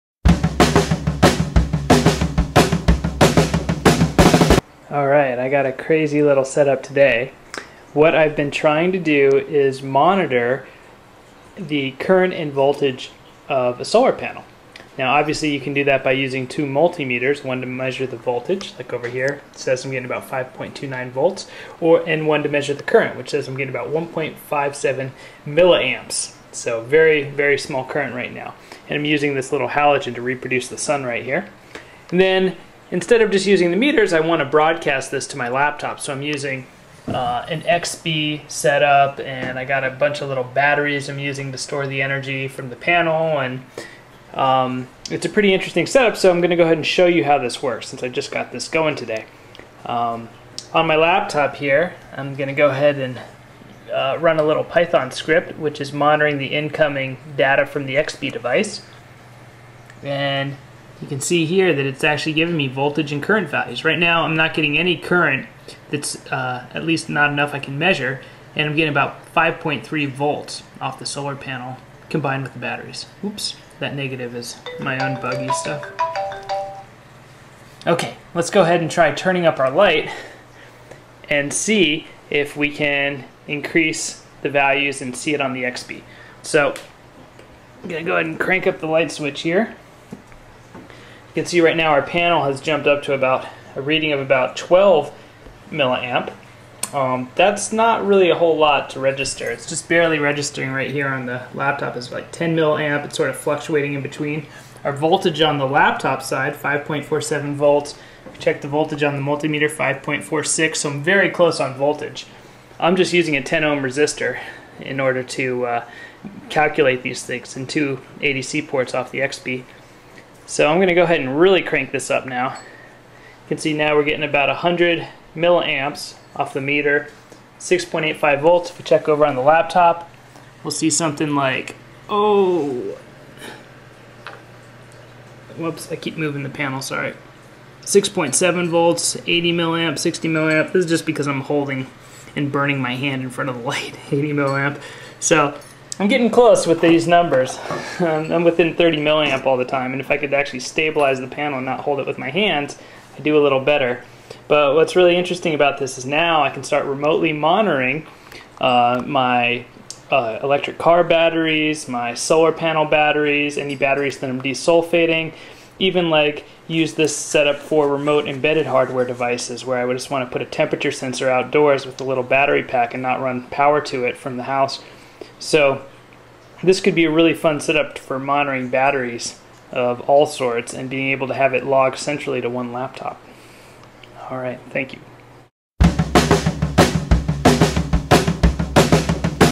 PV-Battery-Xbee
Have you ever wanted to monitor how much voltage/current your small PV solar panels are producing? It is easy enough with a multi-meter, but what if you just want to monitor it remotely and log the data on your computer. This high level overview shows a xbee setup remotely logging a PV panels volts/amps to a mac laptop. It makes use of only 10 ohm resistor and 2 ADC ports. No additional microcontrollers were necessary for this example. The PV solar panel is made by Voltaic Systems and costs only $30 (it peaks at 10.2V @ .133A)